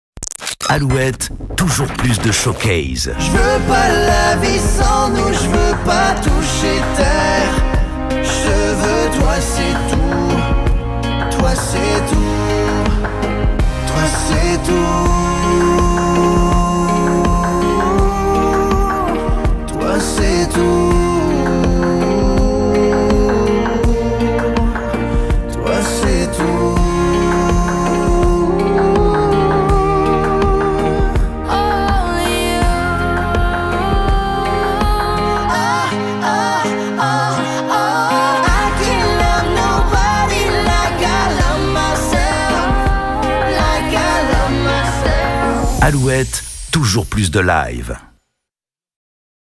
Olivier Dion était en Showcase Alouette !
Revivez en images sur le Showcase Alouette du 9 mai 2018 à Tours avec Olivier Dion !

➡️  Suivez l'actualité de vos artistes préférés sur notre site internet !

➡️  Téléchargez l’application Alouette !